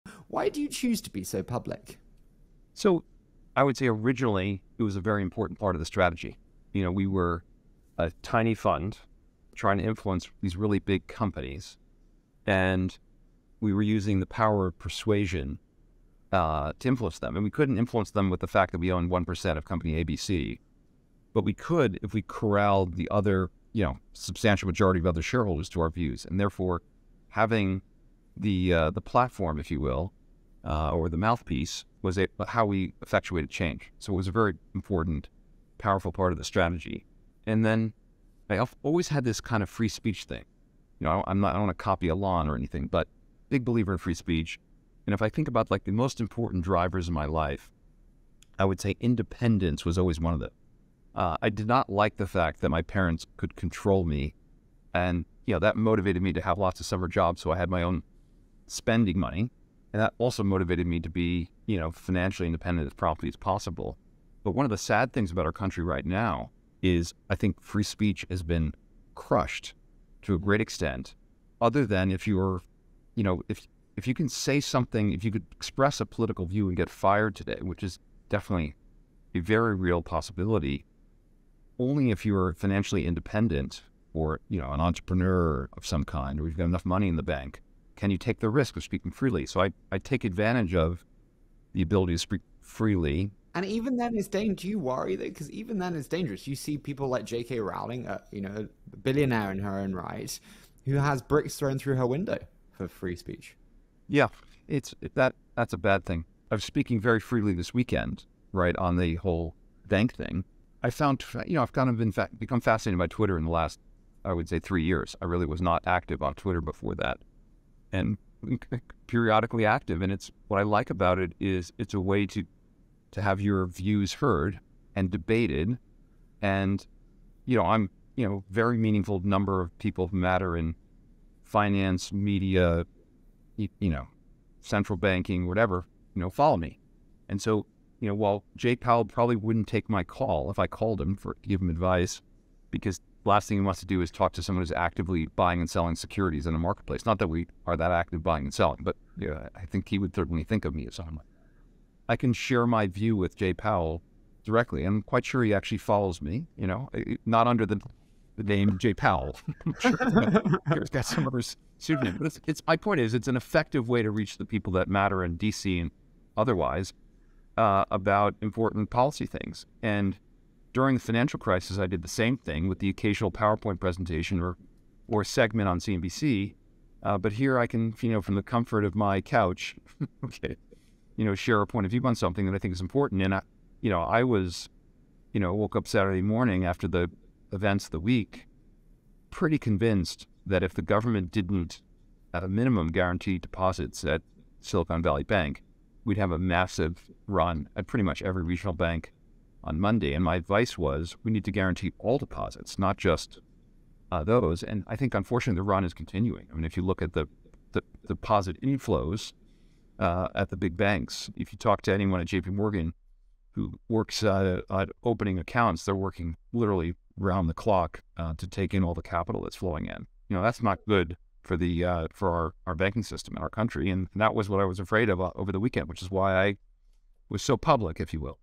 Bill Ackman: Why I Post on Twitter
Bill Ackman is the CEO of Pershing Square Capital Management, L.P., an SEC-registered investment adviser founded in 2003. Pershing Square is a concentrated research-intensive fundamental value investor in long and occasionally short investments in the public markets. Bill is also a member of the board of Universal Music Group N.V. He serves as a member of the Investor Advisory Committee on Financial Markets for the Federal Reserve Bank of New York, and a member of the Board of Dean’s Advisors of the Harvard Business School. Prior to forming Pershing Square, Mr. Ackman co-founded Gotham Partners Management Co., LLC.